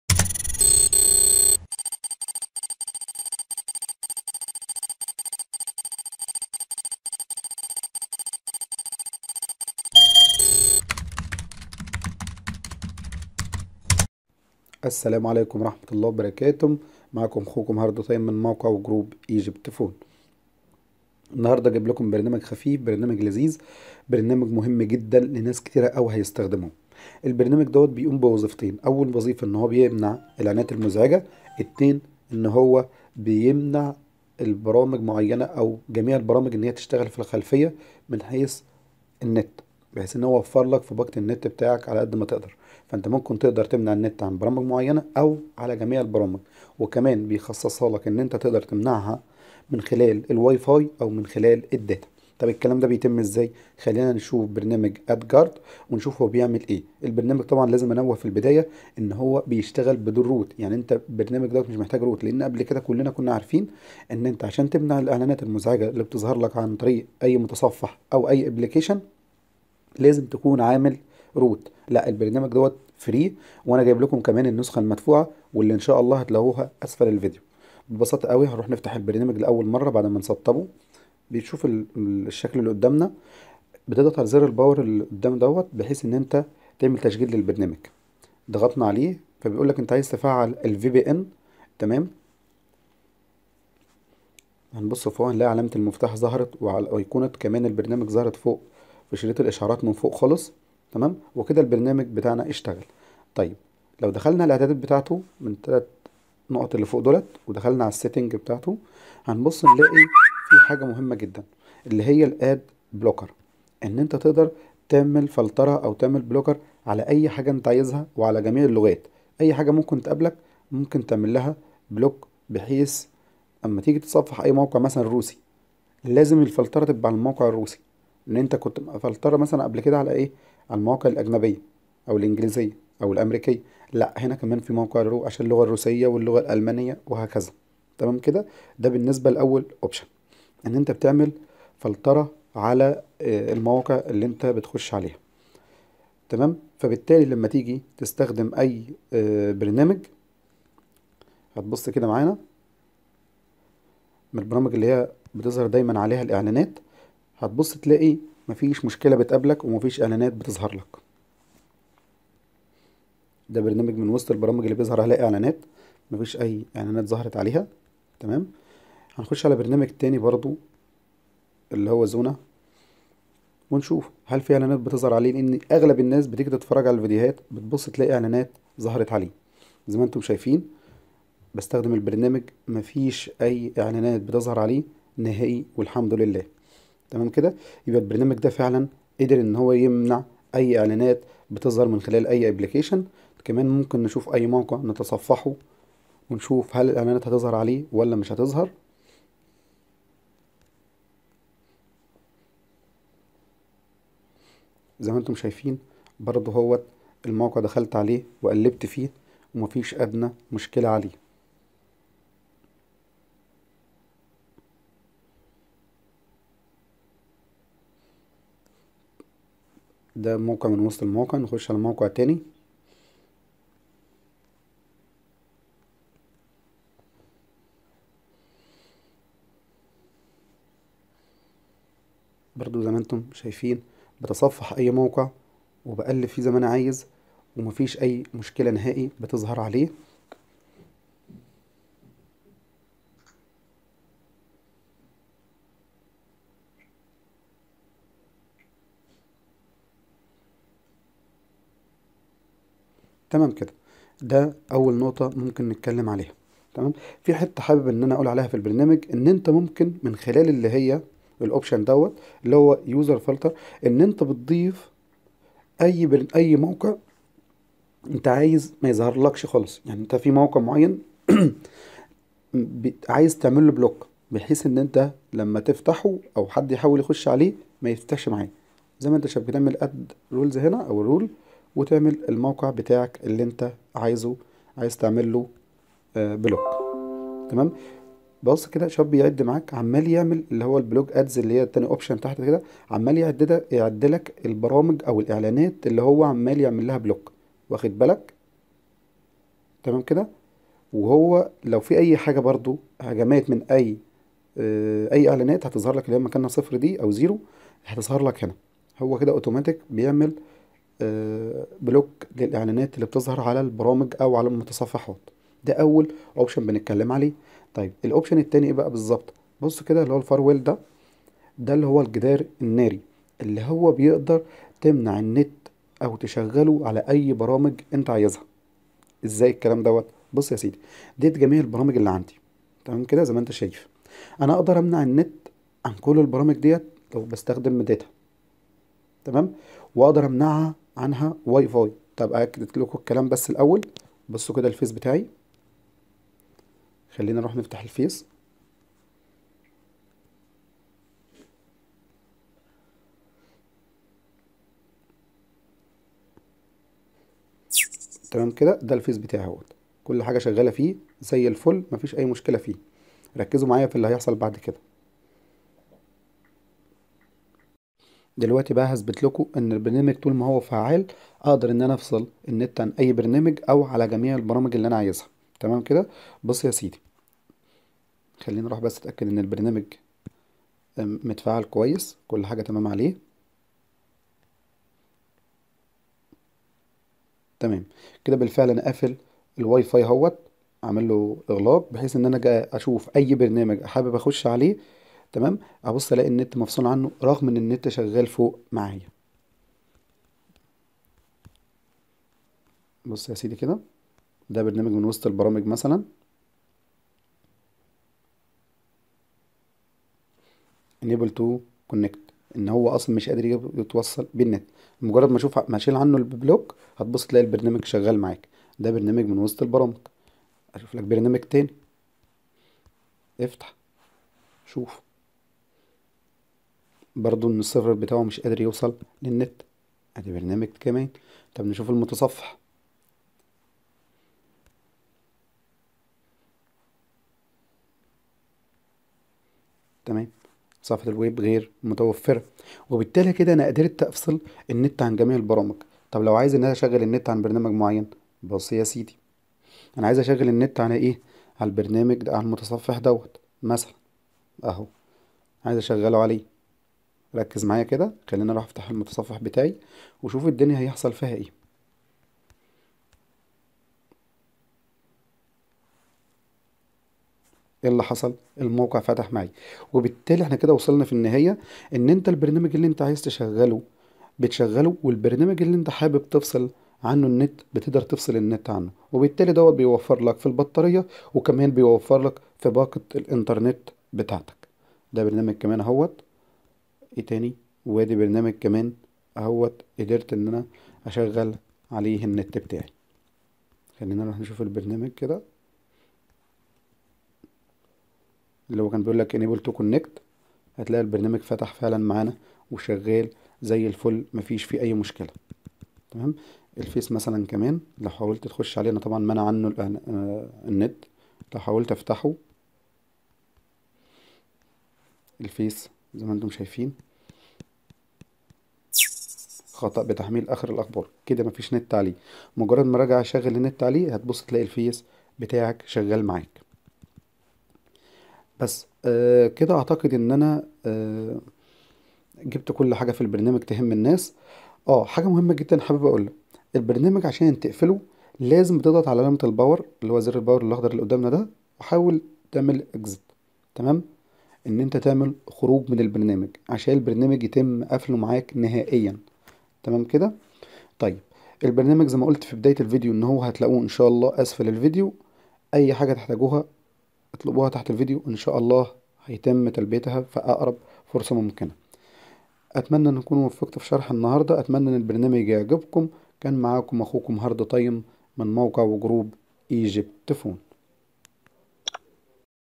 اقوى برنامج لمنع الاعلانات المزعجة على اى موبيل اندرويد بدون روت 2016
اخيرا معا برنامج  AD Guard
هتقدر تتصفح المواقع وتستخم اى برنامج بدون اى
 اعلانات مزعجة كل دة بدون روووت